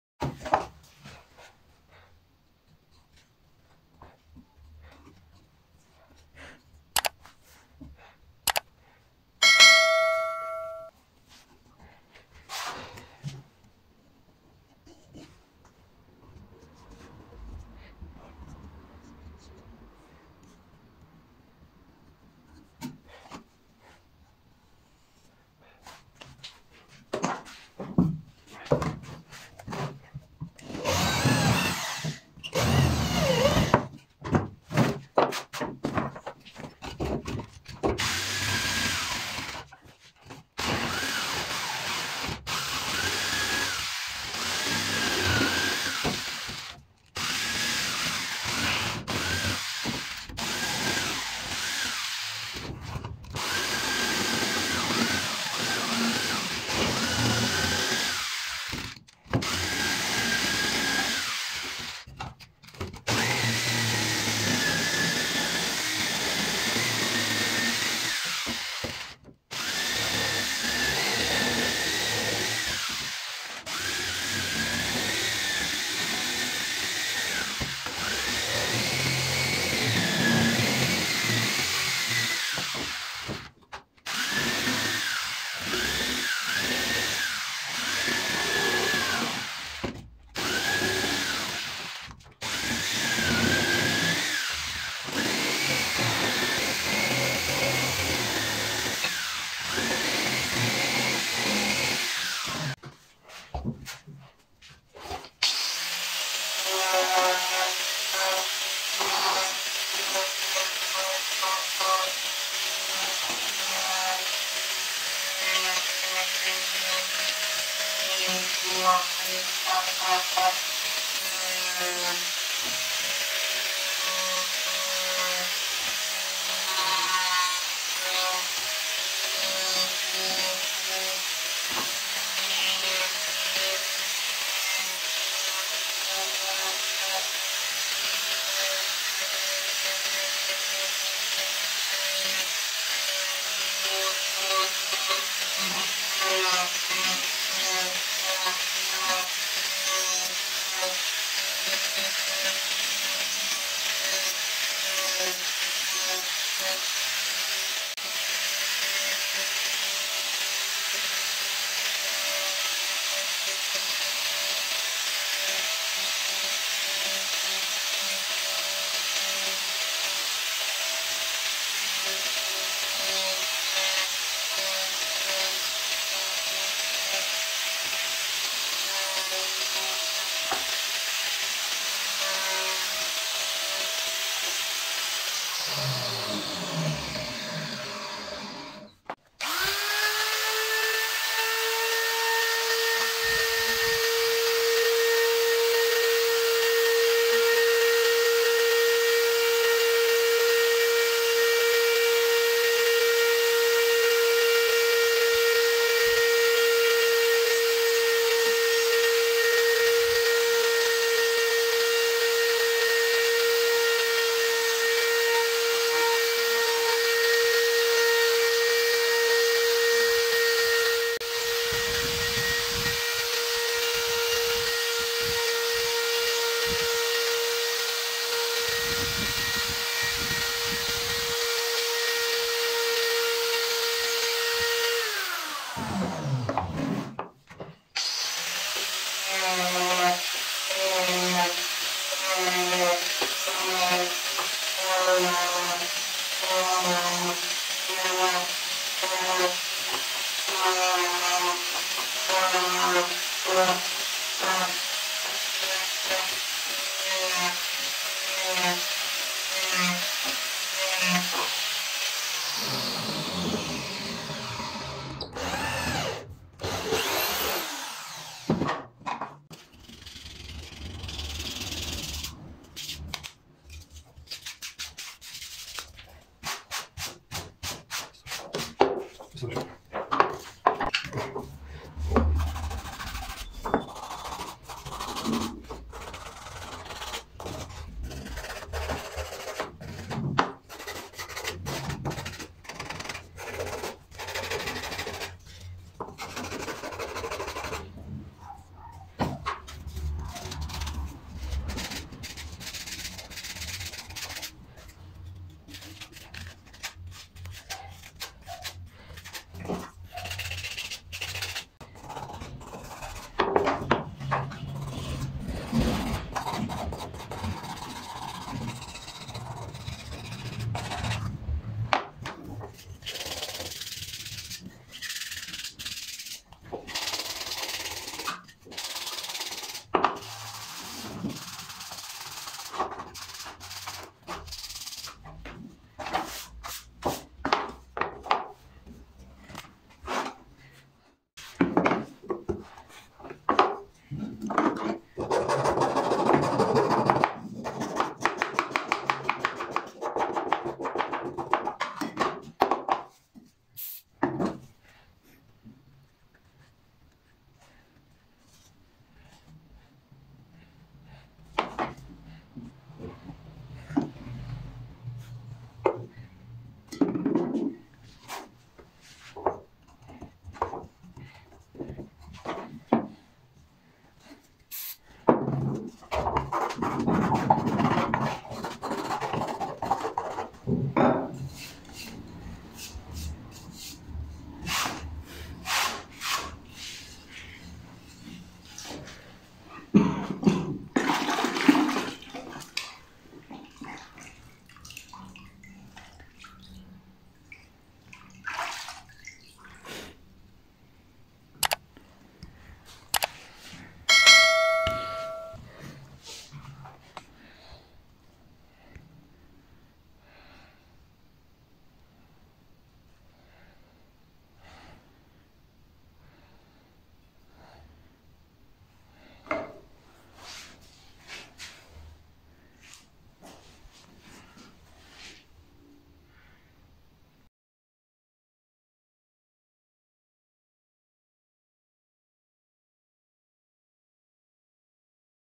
Romantic Sclapture | love Sclapture | dave edmunds
Today i want to show you how to make a Romantic Sclapture.

We need 3 items to make a Romantic Sclapture :

1)wood

2)super glue

3) Romantic Sclapture pattern

Steps to make a Romantic Sclapture :

1)first we draw the pattern of the Romantic Sclapture on a piece of wood.

2)we remove it from wood by using an electric chainsaw.

3)then we sanding it.

"and here you go, your Romantic Sclapture is ready"

We upload videos about woodworking, wood carving and carpentry every day.
if you enjoyed this video, please like and sub🙏 it make us more hard working💪👀